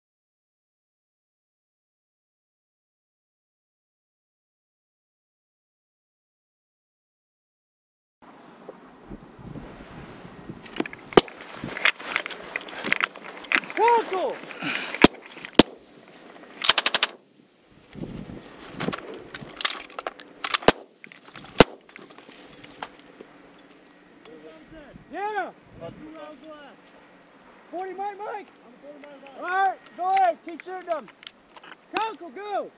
160715-A-BI487-001
B-Roll of U.S. Army infantrymen with Bravo Company, 1st Battalion, 30th Infantry Regiment, 2nd Infantry Brigade Combat Team, 3rd Infantry Division assaulting toward a target during squad live-fire training July 15, 2016 in Thies, Senegal as part of Africa Readiness Training 16. ART16 is a U.S. Army Africa exercise designed to increase U.S. and Senegalese readiness and partnership through combined infantry training and live-fire events. [779S1R7_4SZDM1D06_fc.mov]